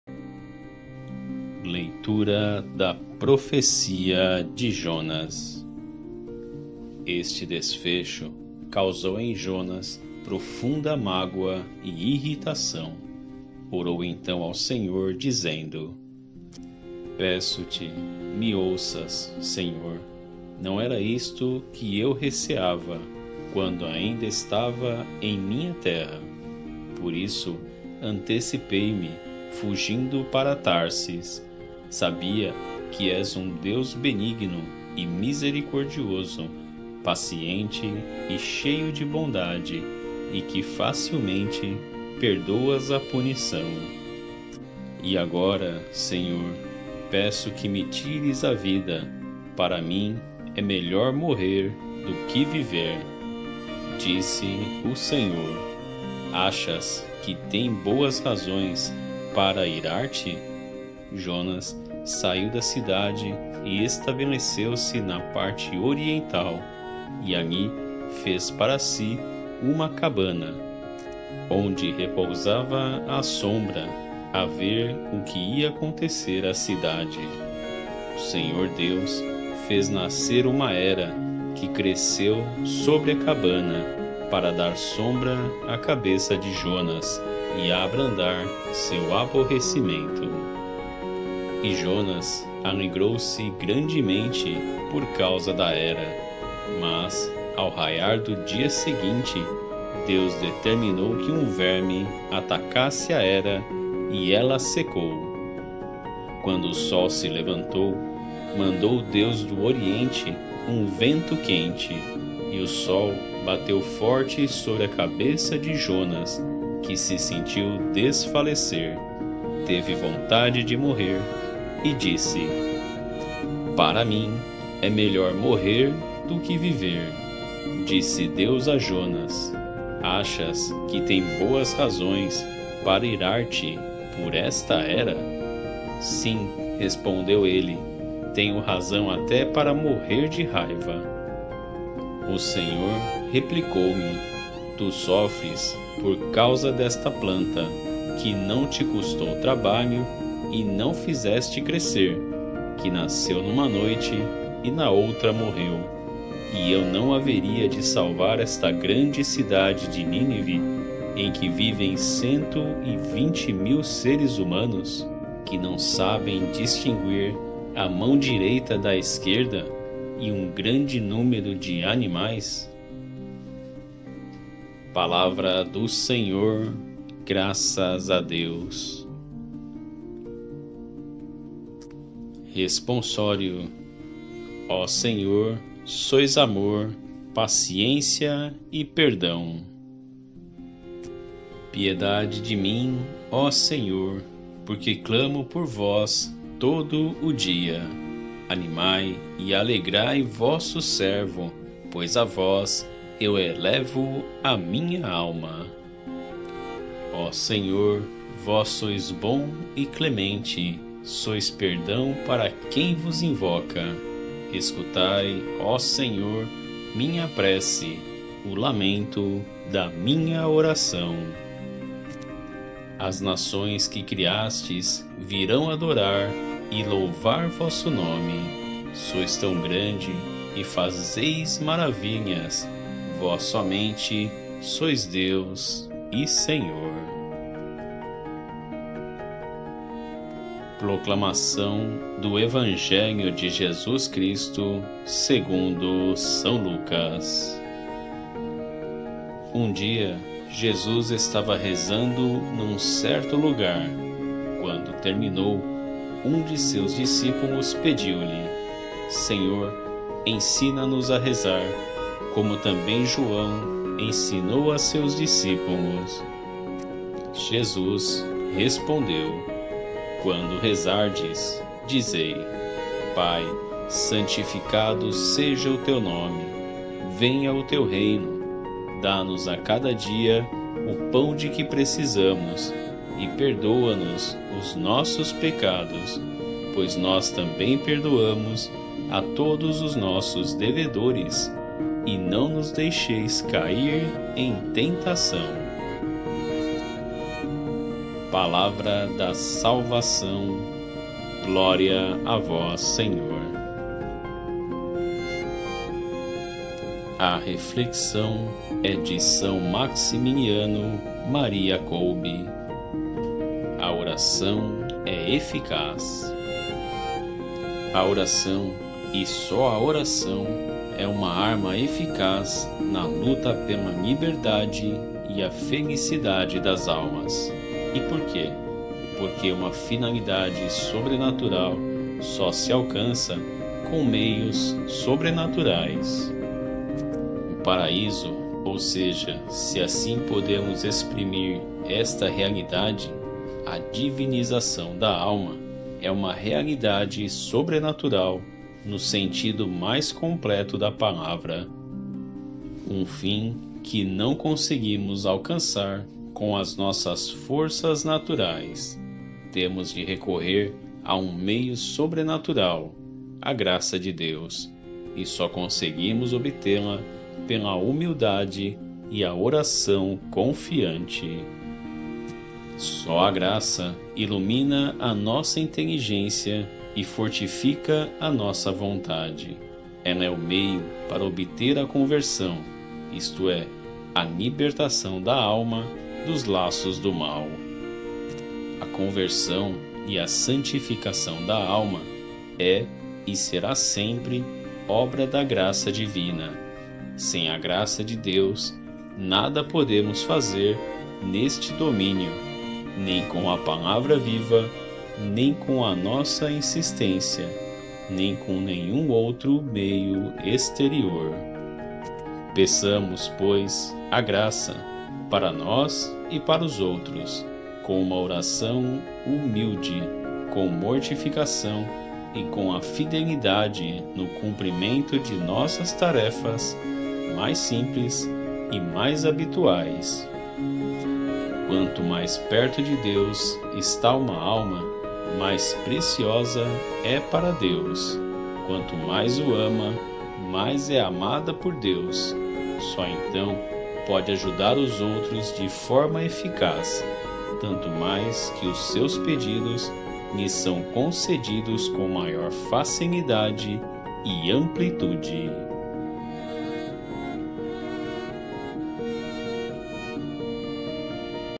Liturgia Diária - Quarta-feira - 06/10/2021 - 27ª Semana do Tempo Comum - com Reflexão
Liturgia Diária

06/10/2021

27ª Semana do Tempo Comum

Primeira Leitura - Jonas 4,1-11
Responsório - Salmo 85
Evangelho - Lucas 11,1-4
Reflexão - São Maximiliano Kolbe (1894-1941)
franciscano, mártir

Colabore com o nosso Canal.
Ajude na Evangelização e na Propagação da Total Consagração à Santíssima Virgem Maria.